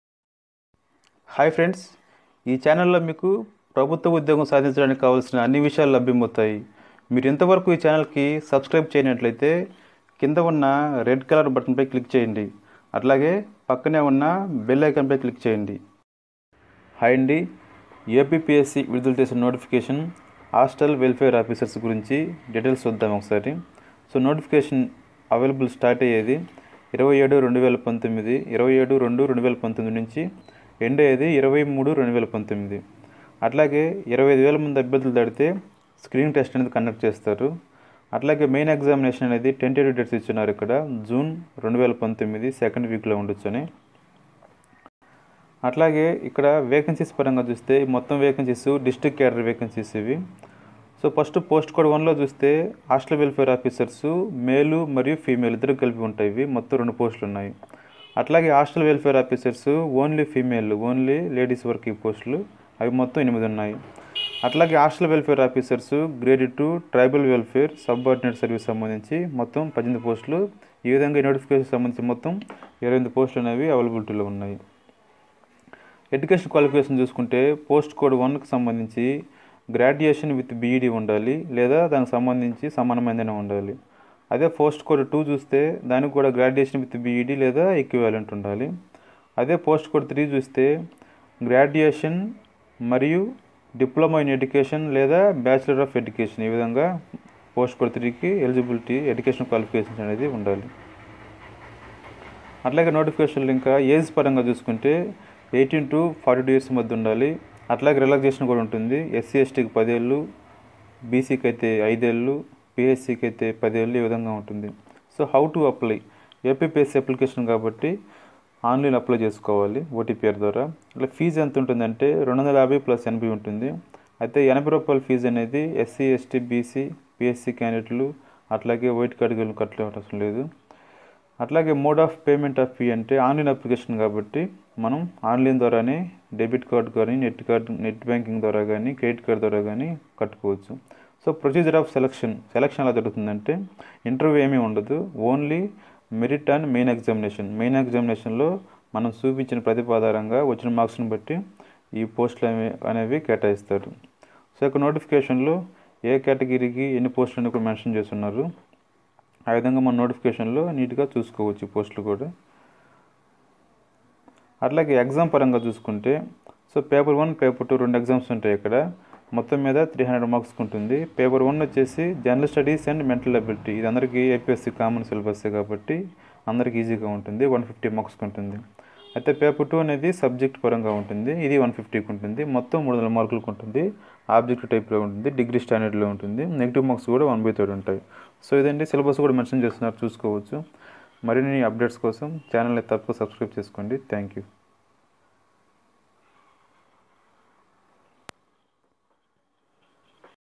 HOSTEL WELFARE OFFICERS-నోటిఫికేషన్ వివరాలు-APPSC
HOSTEL WELFARE OFFICERS GRADE-II IN A.P.TRIBAL WELFARE & B.C WELFARE (MALE & FEMALE)
SUB-SERVICE
Applications are invited online for recruitment to the post of Hostel Welfare Officers
(Grade-II) in A.P. Tribal Welfare and B.C Welfare (Male & Female) Sub Services for a
total of 28 Carried forward vacancies in the scale of pay of Rs.24,440 - 71,510 from
candidates within the age group of 18 - 42 years as on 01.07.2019.